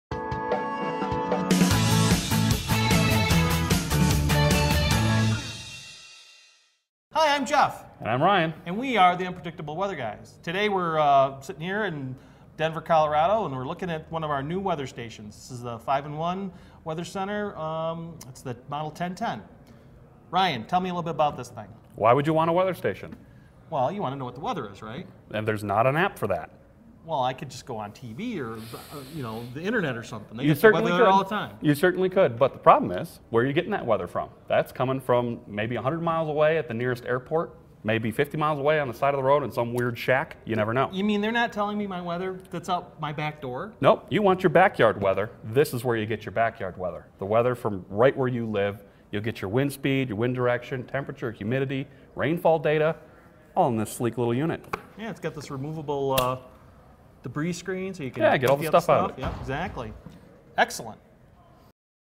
What is a Weather Station?
A weather station is a device that collects and reports weather data from sensors and logs the data on a display. This data can be used to understand current conditions or provide short-term future weather forecasts.

AcuRite weather stations collect and report weather related measurements from a 5-in-1 weather sensor. The 5-in-1 sensor takes weather related measurements and collects atmospheric data right from your backyard. AcuRite weather stations also include a sensor in the indoor display to measure the temperature and humidity indoors.

The 5-in-1 weather sensor includes a barometer, anemometer, wind vane, hygrometer, thermometer and a self-emptying rain gauge all in one sensor. This allows it to track changes in temperature, humidity, wind speed, wind direction, rainfall and barometric pressure to generate a forecast of future weather conditions personalized for your exact location.